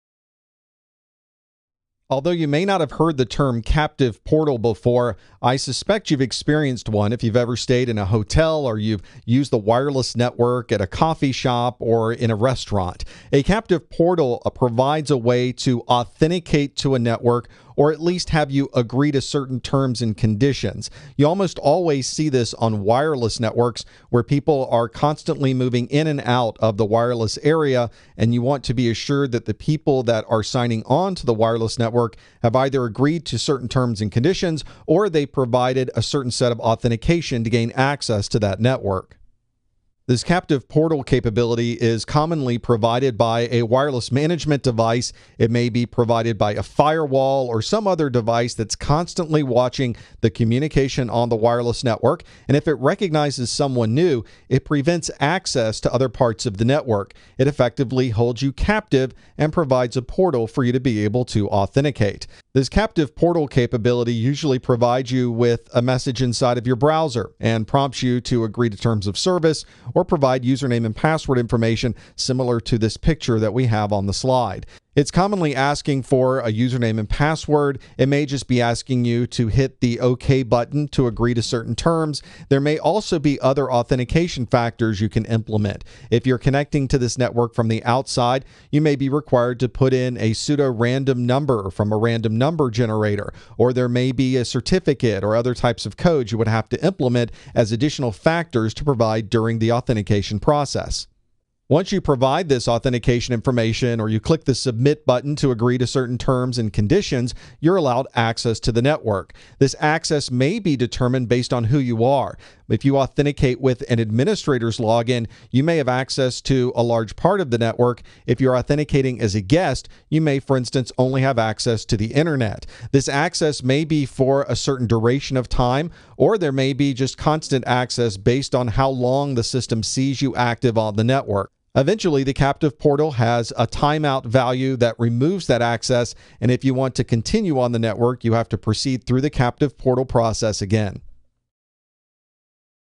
Captive Portals - CompTIA Security+ SY0-401: 1.5
One method of controlling network access is through the use of a captive portal. In this video, you’ll learn how administrators use captive portals to increase the security of the network.